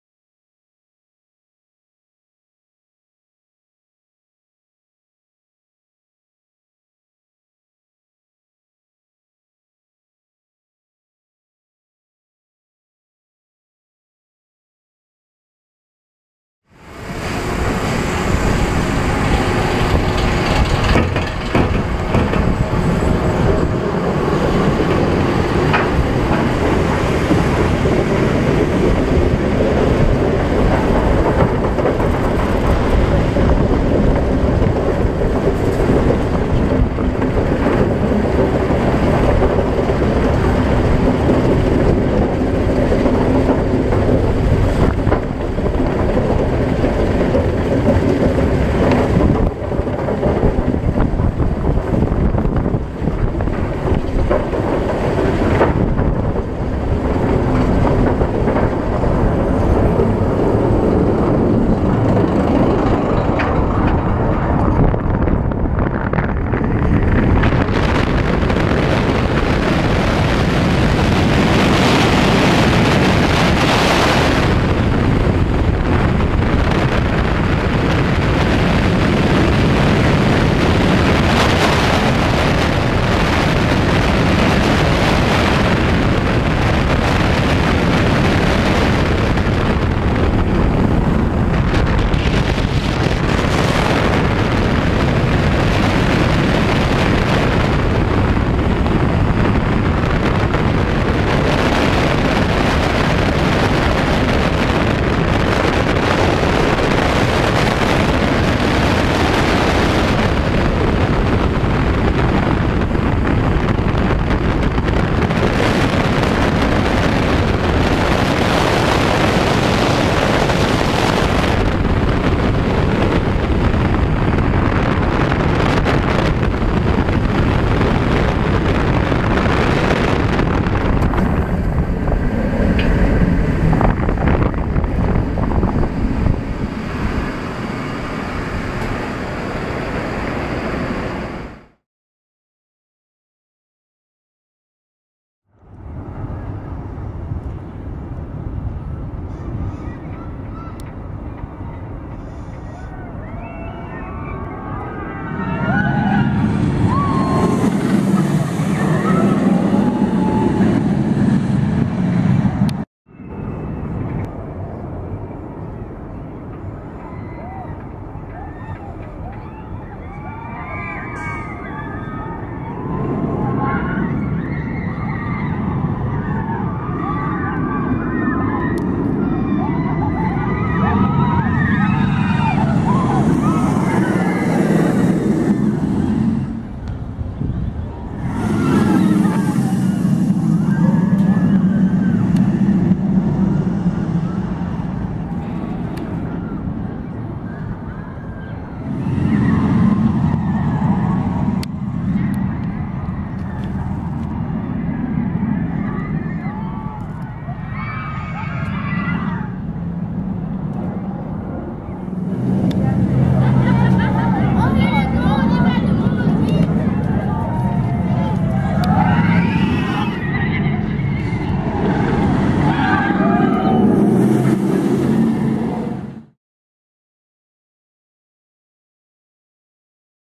Fury 325 - Carowinds, on/off ride
Maak een ritje in Fury 325.
Fury 325 is een stalen achtbaan geproduceerd door B&M.
De baan is geopend op de datum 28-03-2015.

Uitgebreide specificaties

Model - Hyper Coaster
Lift/launch system - Chain lift hill
Hoogten - 99 m
Drop - 98 m
Lengten  - 2,012 m
Snelheid - 153 km/h
Inversions - 0
Max vertikale drop  -  81°
Capaciteit - 1,470 rider per uur